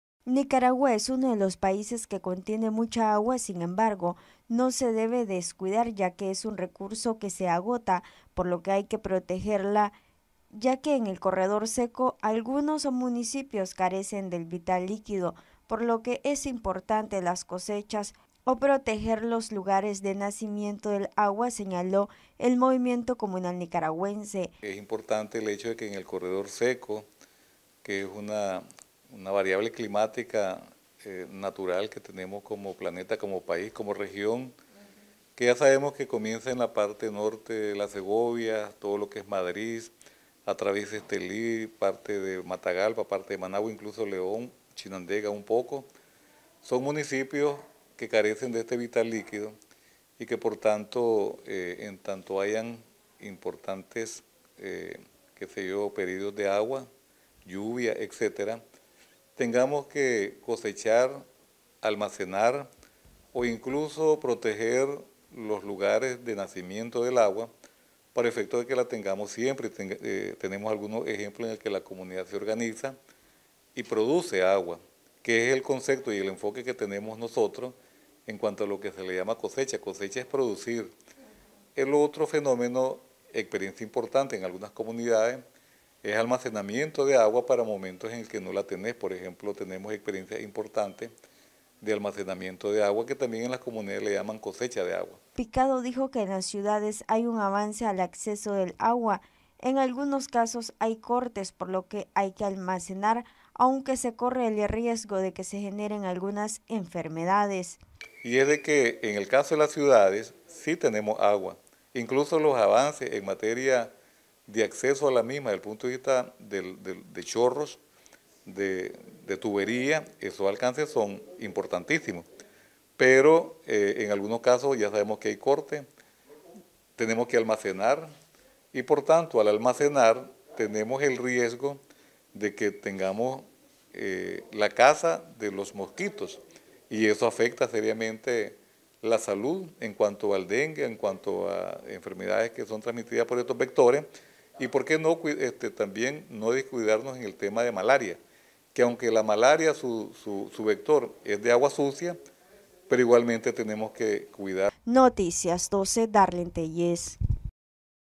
Noticias12Nicaragua Principales beneficios de cosecha de agua en las comunidades del corredor seco
Principales beneficios de cosecha de agua en las comunidades del corredor seco